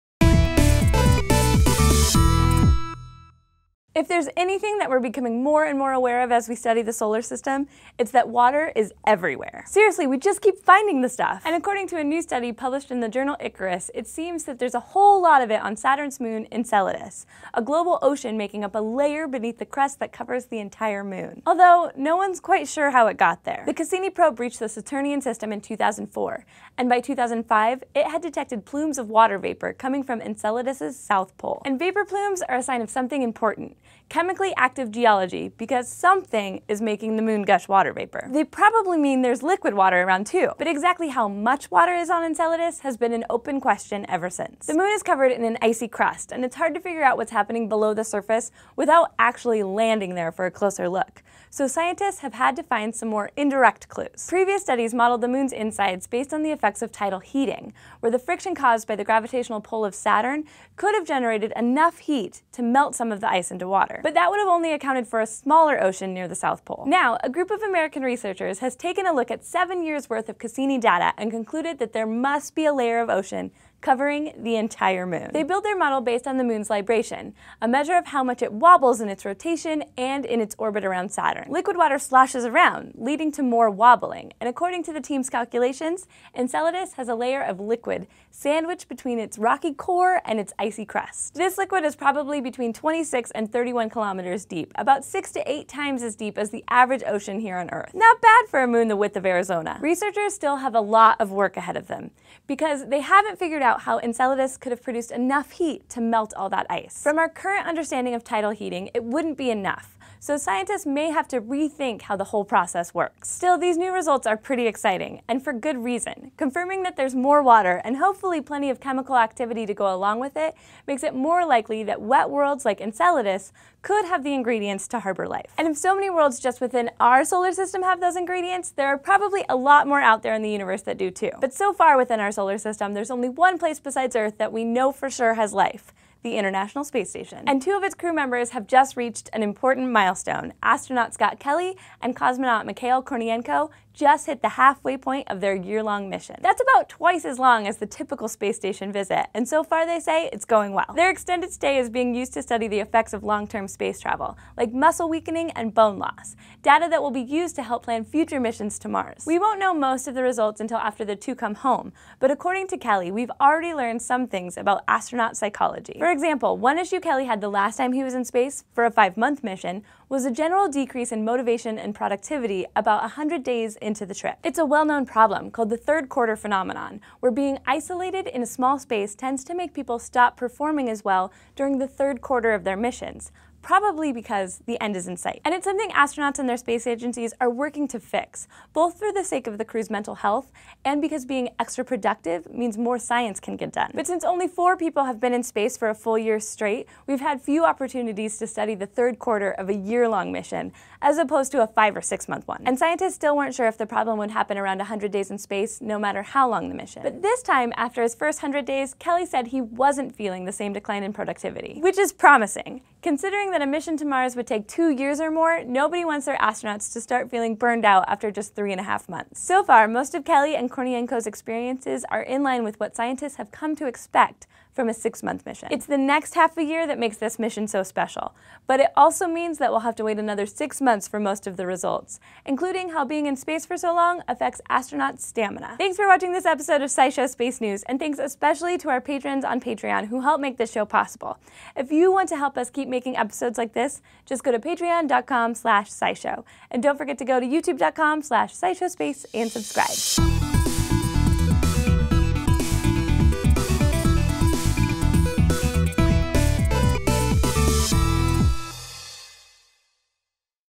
The Ocean on Enceladus is Really Big
This week on SciShow Space News, Saturn’s moon Enceladus probably has a giant ocean covering the entire world. And the year-long mission to the International Space Station just hit its halfway point!